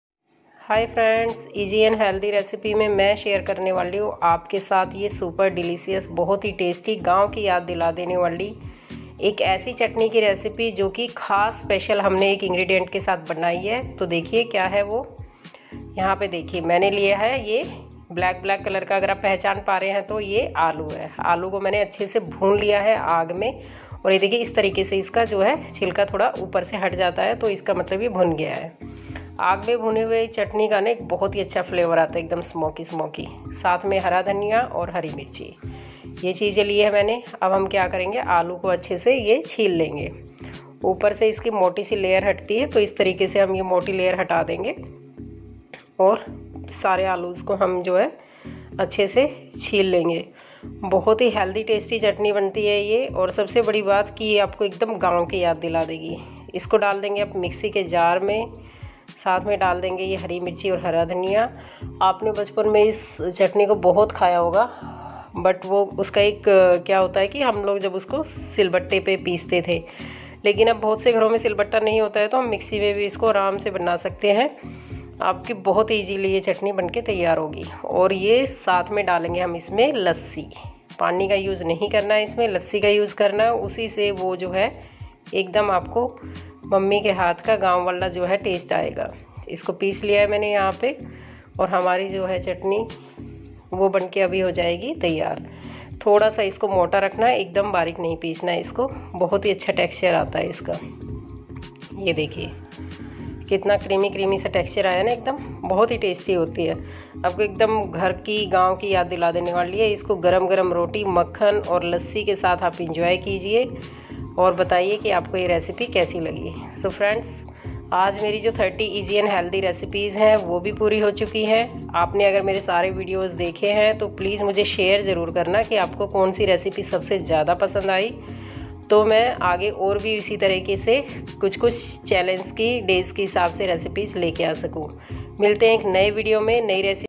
30/30 easy n healthy recipe बहुत ही शानदार भुने हुए आलू की चटनी रेसिपी।roasted potato chutney
दोस्तों आज मैं शेयर कर रही हूं आपके साथ भुने हुए आलू की बहुत ही जबरदस्त चटनी की रेसिपी जो कि आपको एकदम गांव की आपके बचपन की याद दिला देगी आप इस रेसिपी को जरूर ट्राई करें और शेयर करें अपना फीडबैक कमेंट बॉक्स में ।